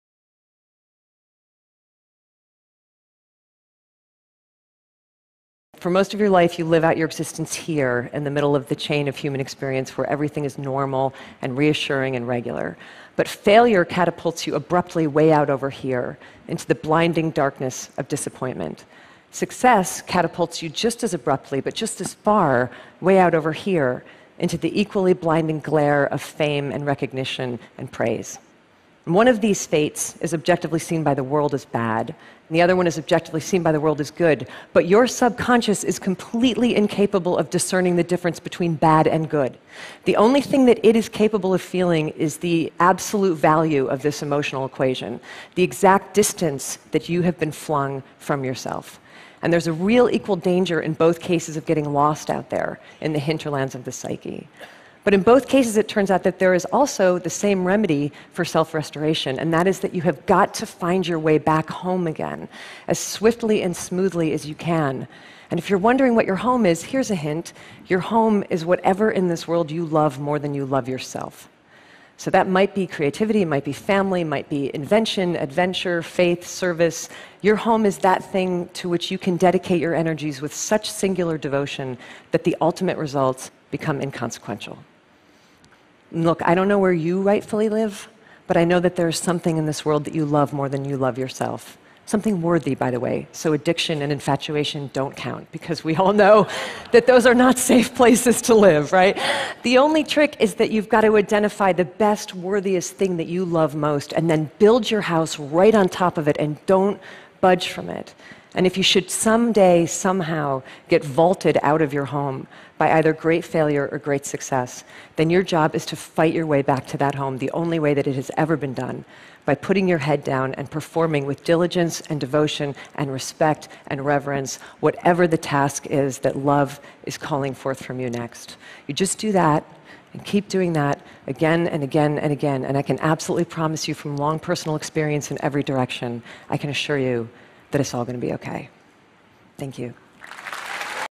Success, Failure and the Drive to Keep Creating - Elizabeth Gilbert
"You have got to find your way back home, which whatever in this world you love more than you love yourself".